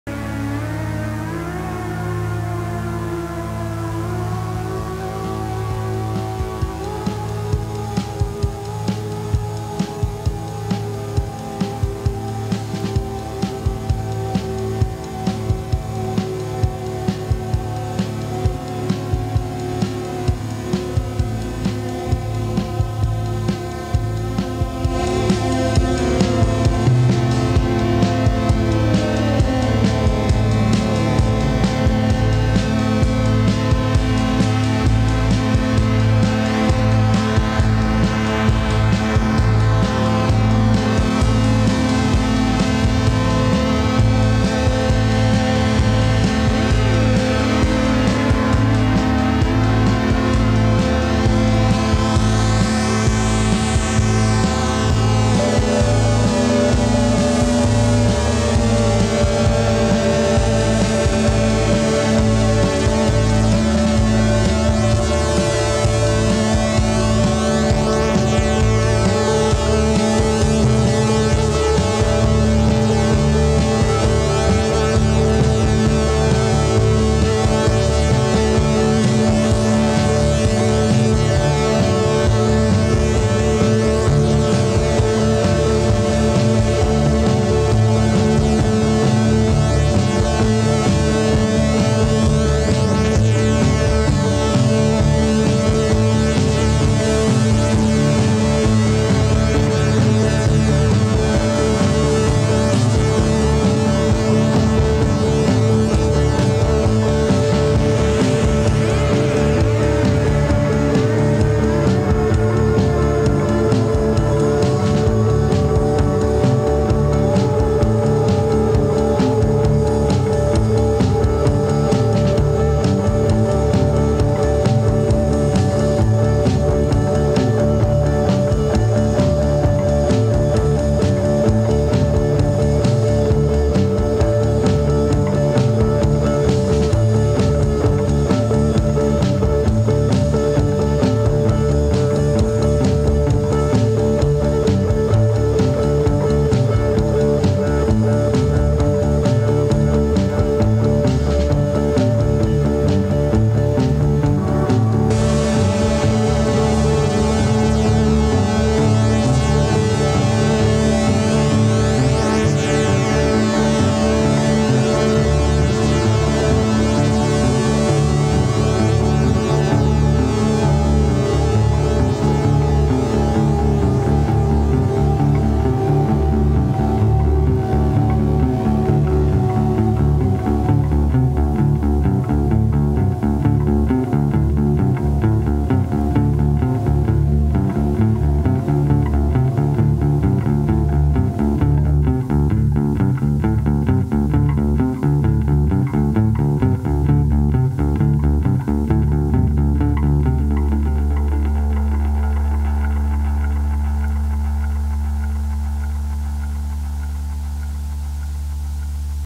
Megascops, in the Cube.
performing inside 'the cube' at my studio.
recorded on cassette (my computer was busy running the cube)